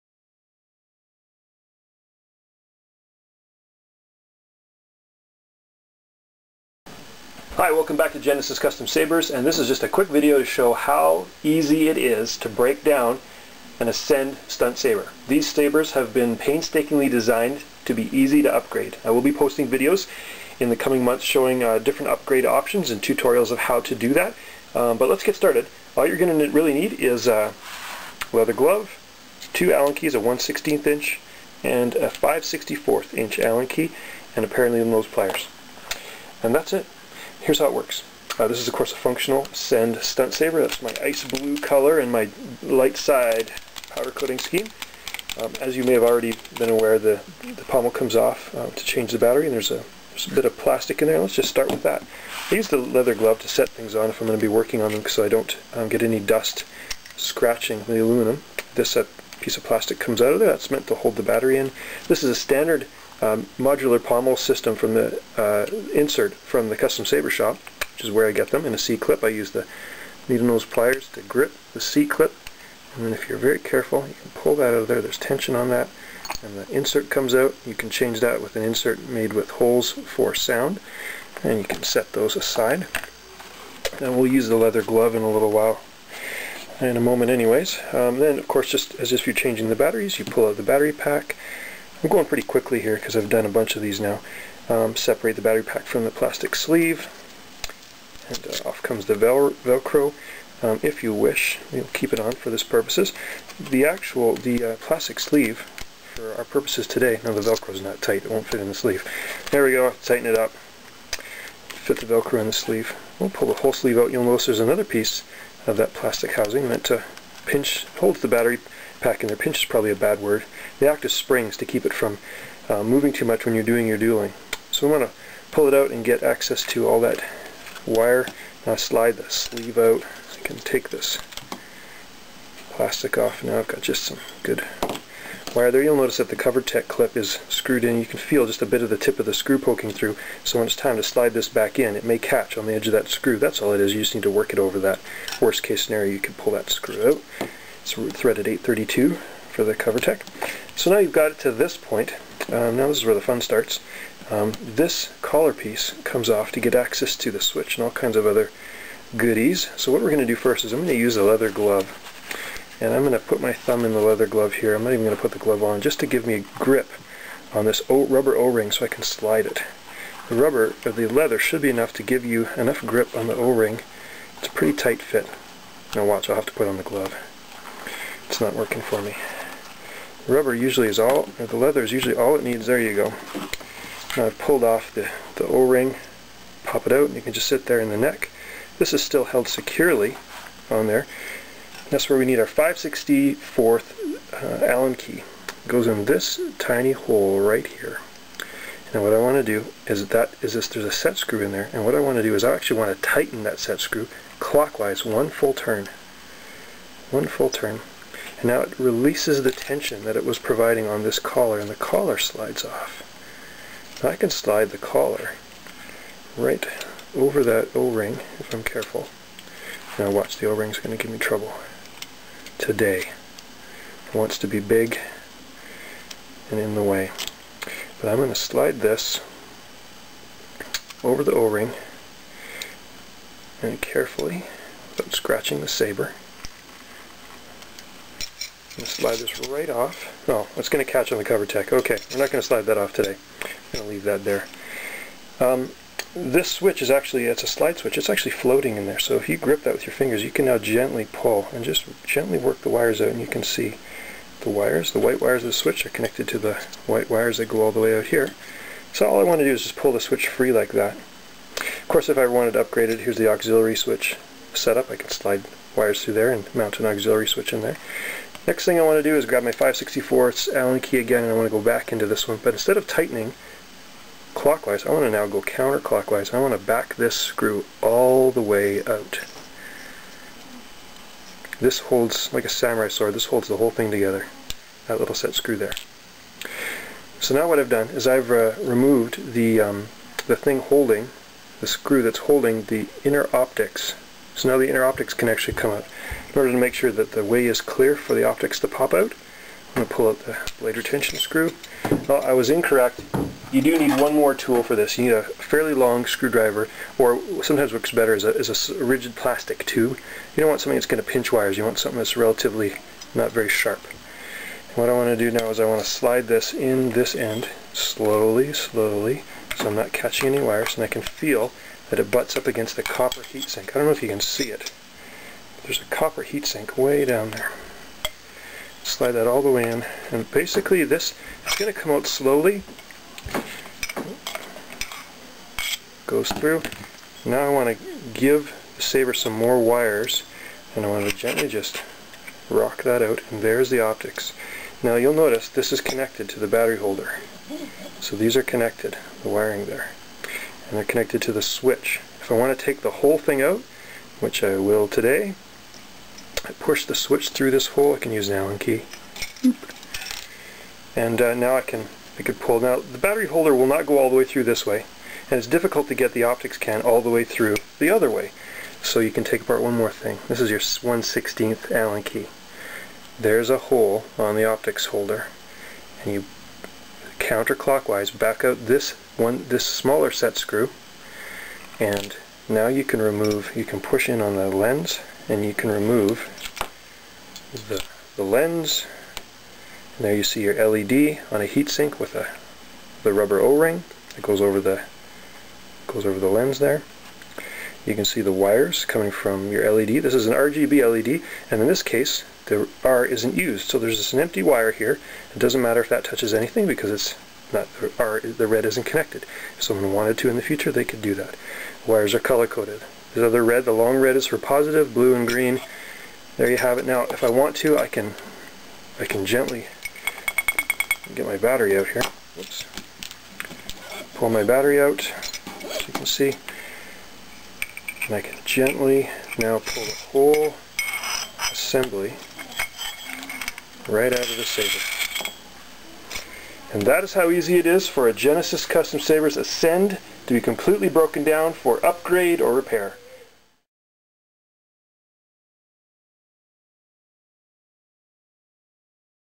Ascend Stripped Down
These procedures are meant for a sabersmith of medium experience. These things will void your Ascend Limited warranty. This is how you can strip down the Ascend saber for upgrade.